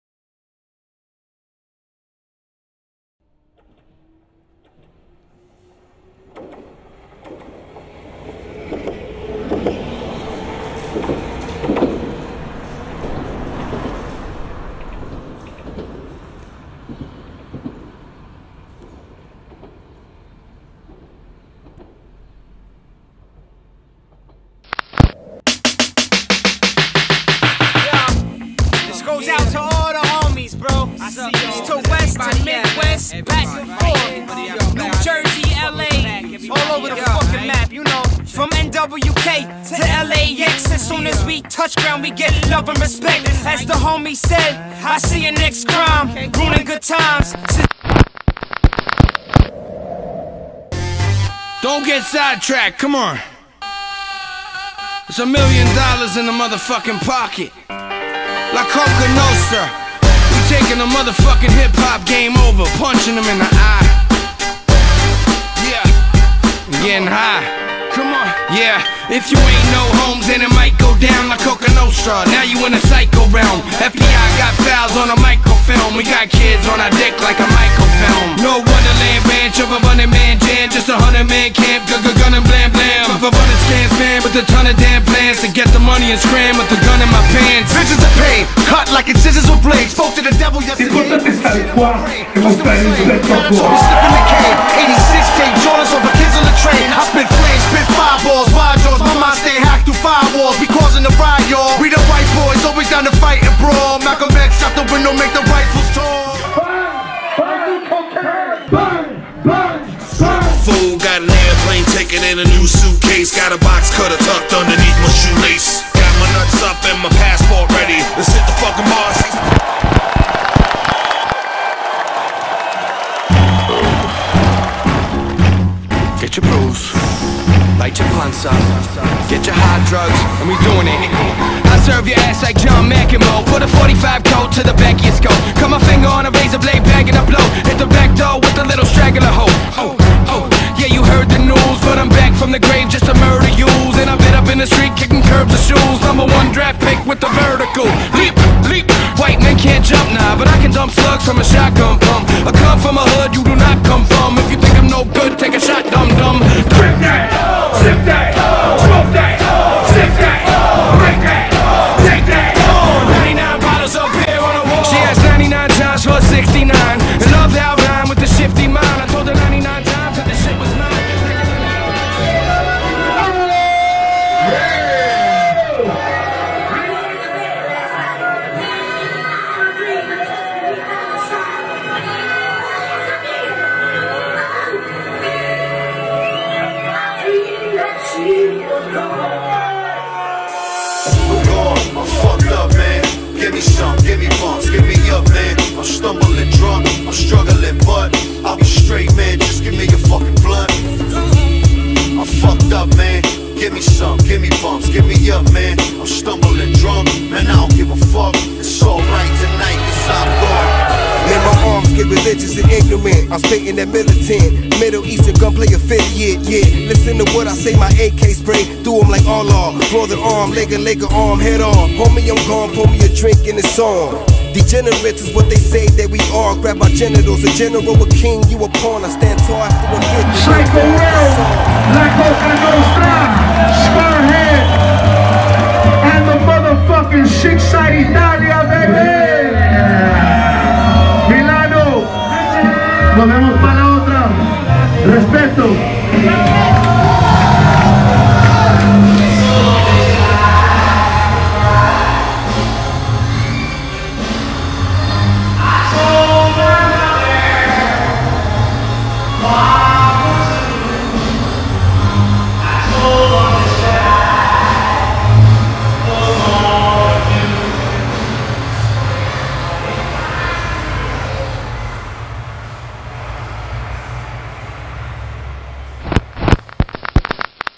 The Violent Voices Tour 2015 ( Italy )
Propaganda & 747 presentano :
The Violent Voices /
Sick Jacken, Slaine & Danny Diablo + Egreen + Aban + Deal Pacino + The Night Skinny + guests.